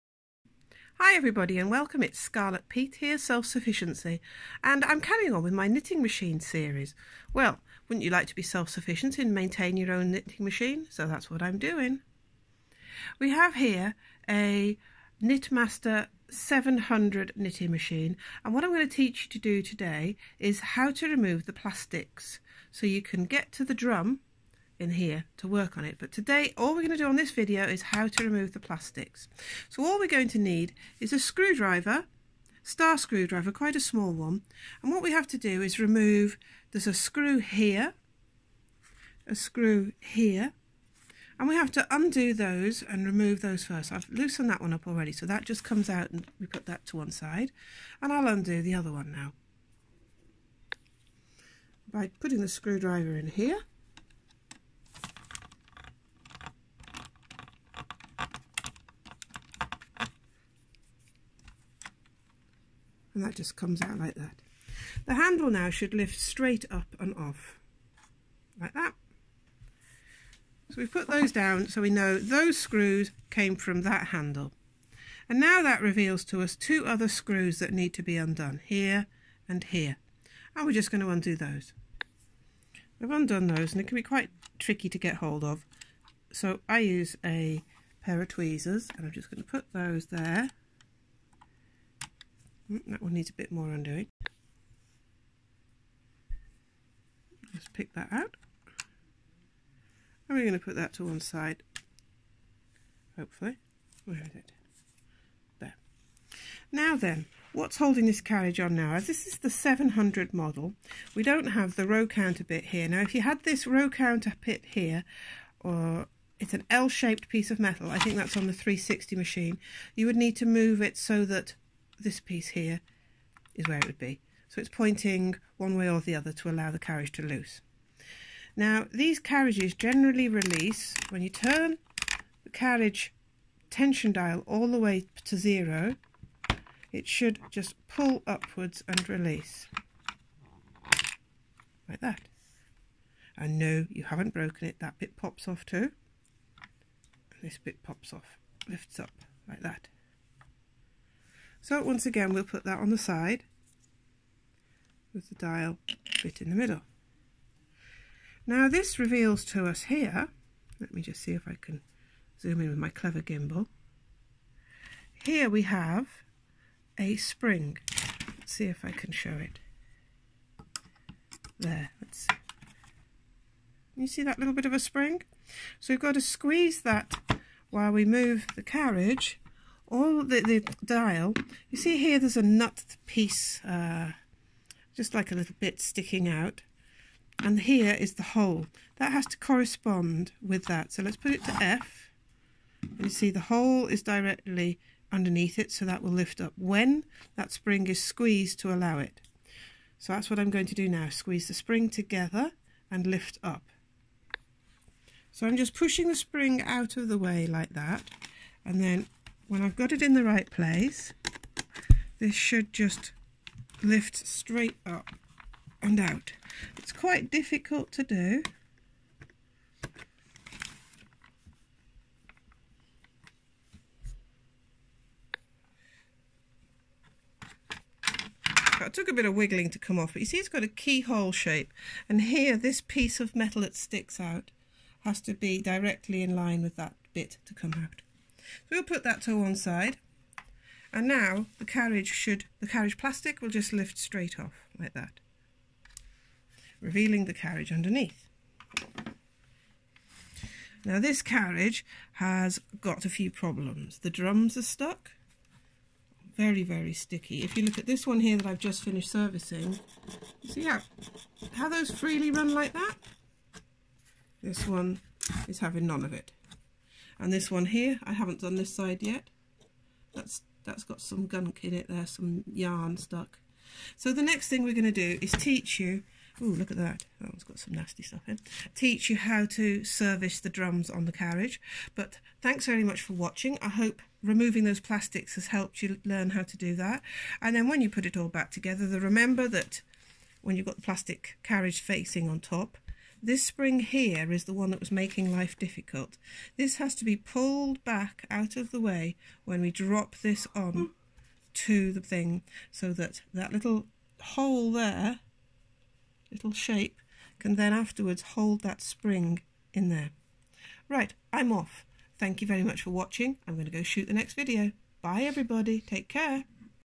Removing the plastics from knitmaster 700 knitting machine carriage.
Part of my step by step guide to servicing your knitmaster machine.

Please take care and follow my guidelines. but remember it can go wrong. You can possibly break things. but if you want to give it a go. Here's how I do it .